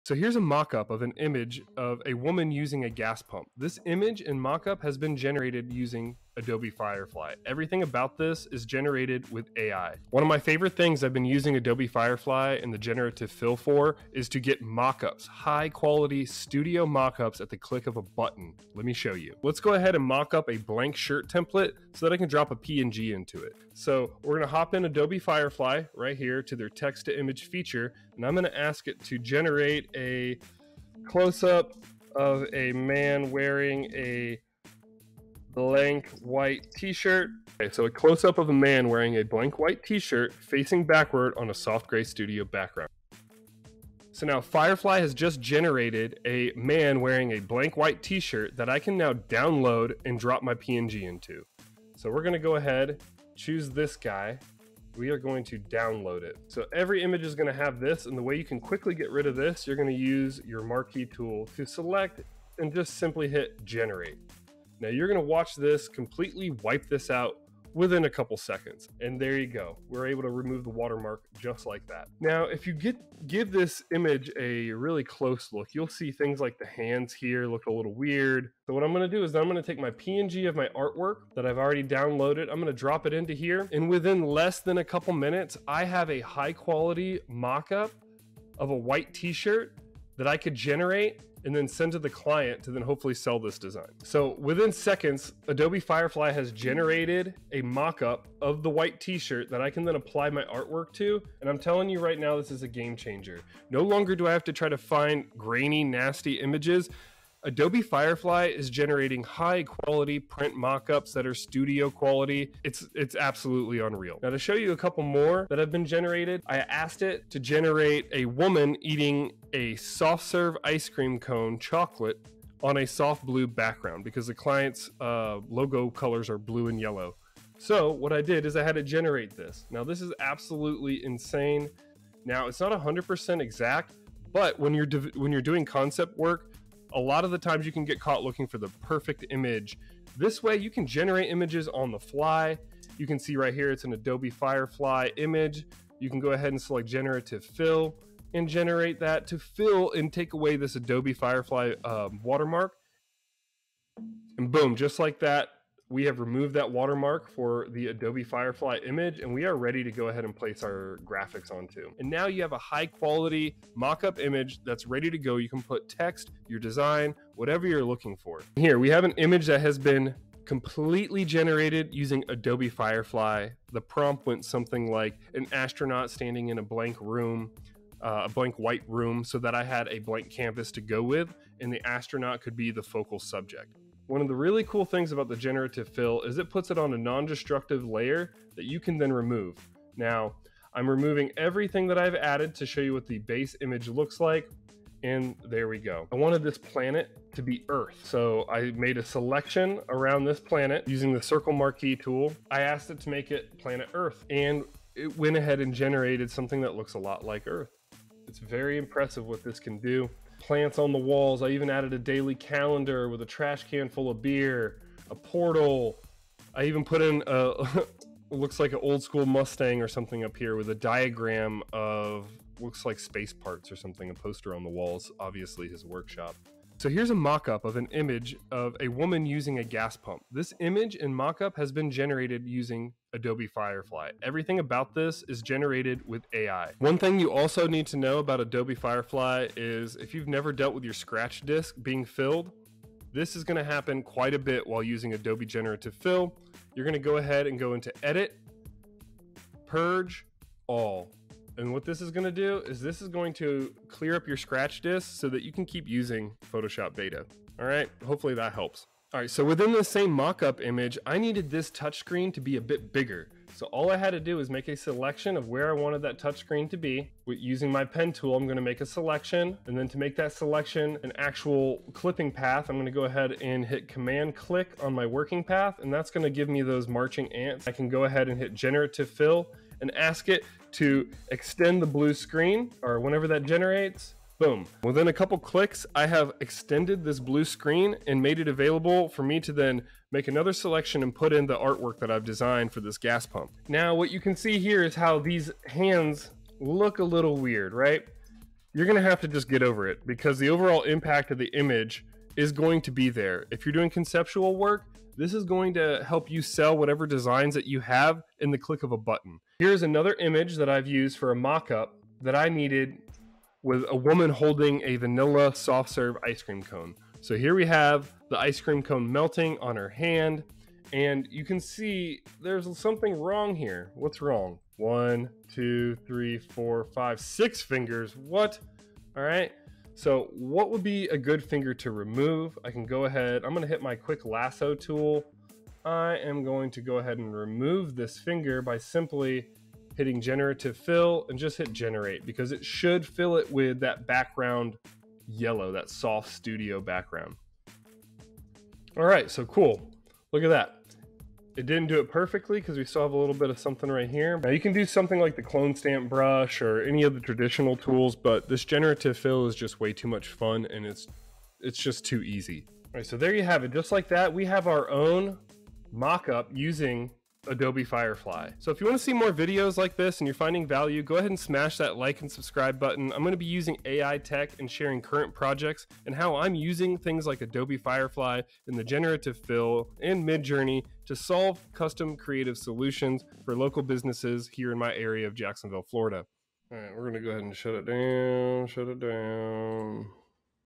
How to Use Adobe Firefly for Quick Photo Mockups
I can't tell you how much time I waste searching for the right image when working creating concepts or mock up to display designs and sell to clients. Adobe firefly is next level is how accurate and quick it can produce photo studio quality images. Firefly will certainly change the my creative workflow as it continues to roll out the new features.